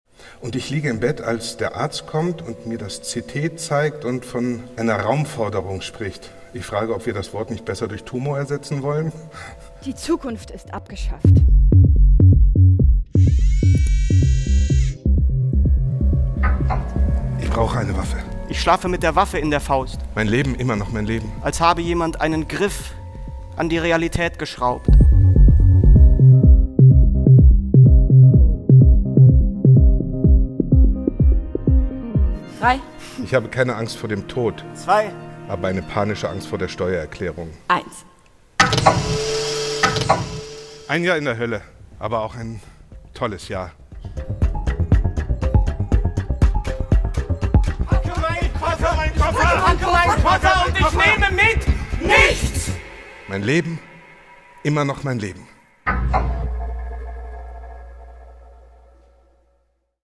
Arbeit und Struktur — von Wolfgang Herrndorf — Trailer
Arbeit und Struktur — von Wolfgang Herrndorf — in einer Fassung von Robert Koall — Uraufführung am 9. September 2023 — Schauspielhaus, Kleines Haus — Schauspiel

Als sich der Schriftsteller Wolfgang Herrndorf am 26. August 2013 das Leben nahm, starb mit ihm einer der begabtesten, wichtigsten und wirkungsmächtigsten Autor:innen des noch jungen 21. Jahrhunderts. Seine literarische Schaffensphase umfasste kaum mehr als zehn Jahre, und es sind nur wenige Bücher, die er hinterlassen hat. Die aber haben ein Schwergewicht, das seinesgleichen sucht. Sein Thriller »Sand« gewann den Preis der Leipziger Buchmesse, »Tschick« wurde innerhalb weniger Jahre zu einem millionenfach aufgelegten Klassiker der neuen deutschen Literatur (und des Deutschunterrichts). Und über »Arbeit und Struktur« schrieb der Literaturwissenschaftler Michael Maar: »Bei aller Hochschätzung für Herrndorfs Romane – sein Blog ›Arbeit und Struktur‹ steht ihnen an literarischem Rang nicht nach. Es gibt in der Geschichte der Tagebücher nichts, was ihm gleichkäme an Takt, Wärme, dunklem Witz, Sarkasmus und stillem Grauen.« »Arbeit und Struktur« ist Herrndorfs Tagebuch seines angekündigten Todes. Nachdem bei ihm 2010 ein bösartiger Hirntumor diagnostiziert worden war, stürzte er sich in einen unvergleichlichen Schaffensrausch. Er schrieb weiter Bücher im Wettlauf gegen den Tod und dachte zugleich in seinem Internet-Blog öffentlich über das Leben, das Sterben, die Liebe, die Kunst, die Freundschaft, das Weltall und den ganzen Rest nach. Über drei Jahre währte dieses literarische Projekt, dem täglich zehntausende Leser:innen folgten – bis zu seinem unvermeidlichen Ende, das der Autor selbst setzte.

Zum 10. Todestag inszeniert Adrian Figueroa (»Biedermann und die Brandstifter«, »Das Tribunal«) Herrndorfs großen Text über den Abschied von der Welt.